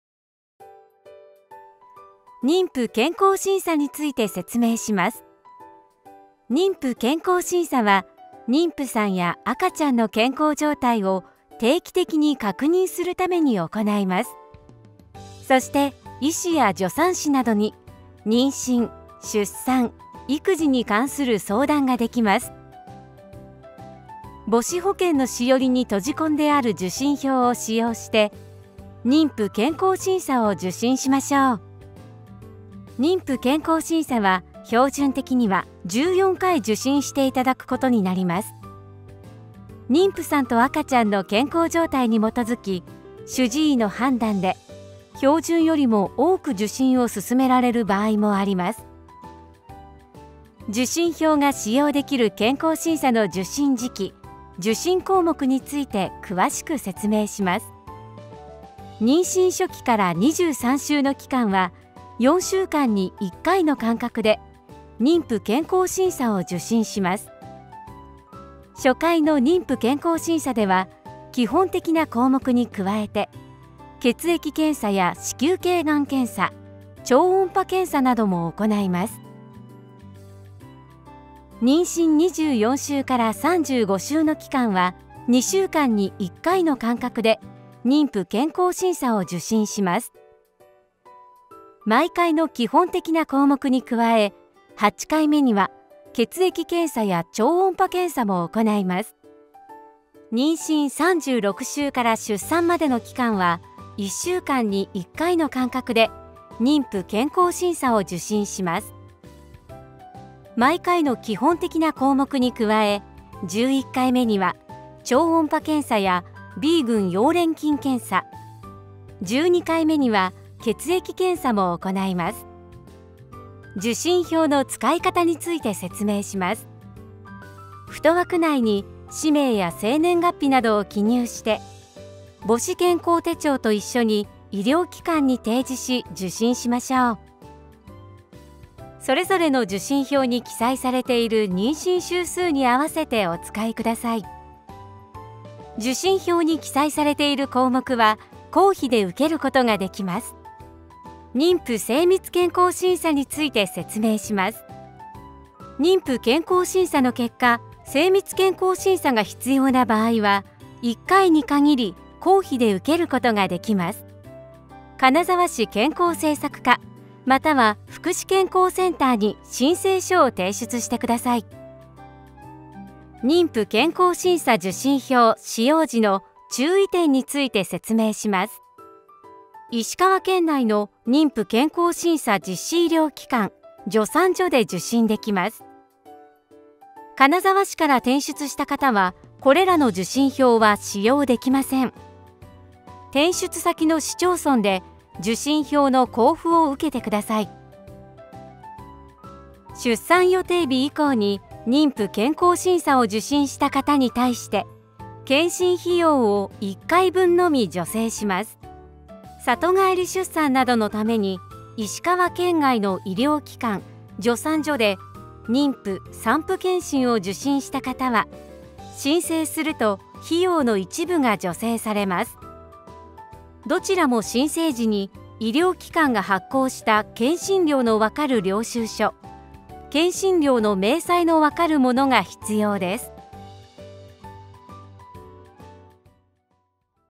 妊婦健康診査
金沢市の母子保健制度等の紹介です。
～②妊婦健康診査を受けましょう～